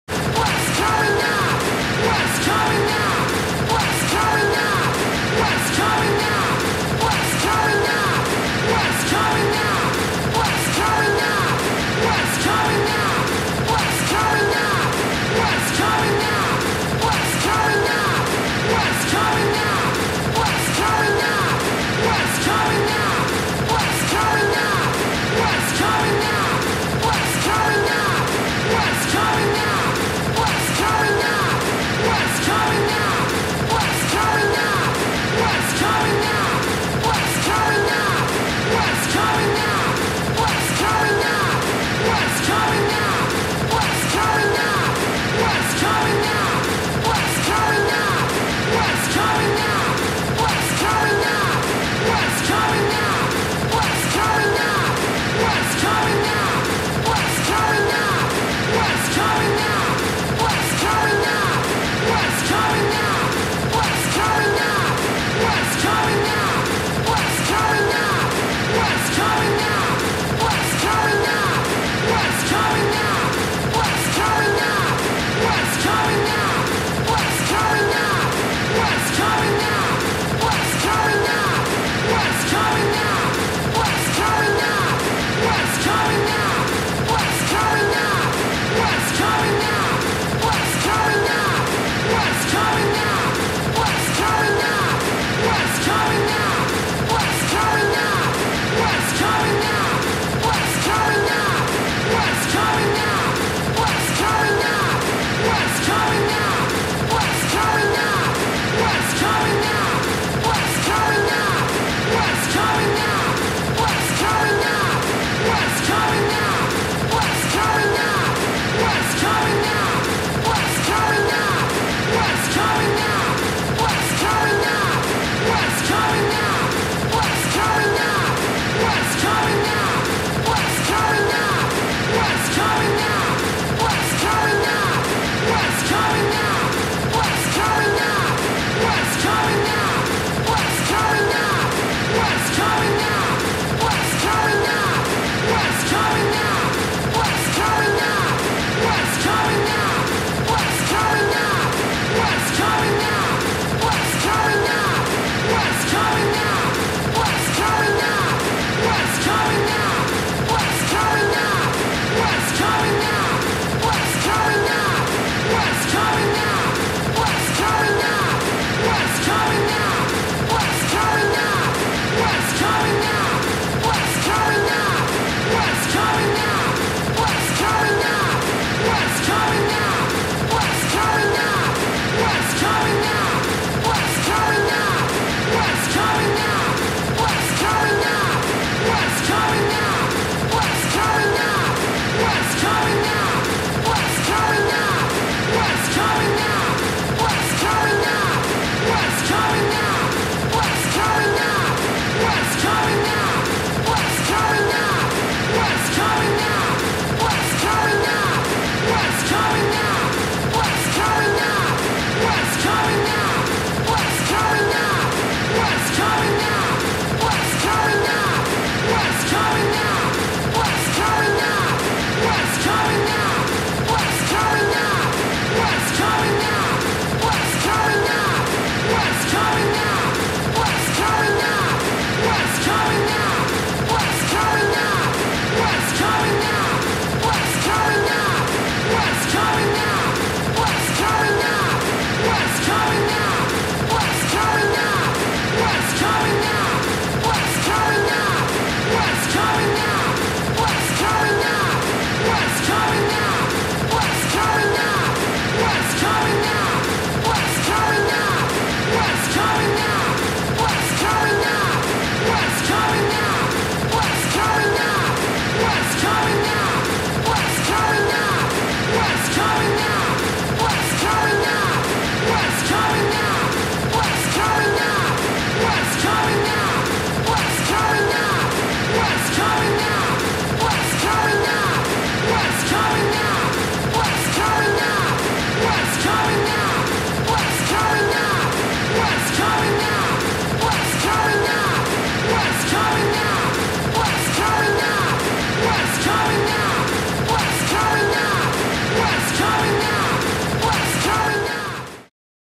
【高音質】リベラドライバー 待機音 完全版
玩具版はこっち。実は本編で一瞬流れてた待機音
雑音もあるので不完全高音質としてます

再生リスト限定公開だった動画を全体公開しました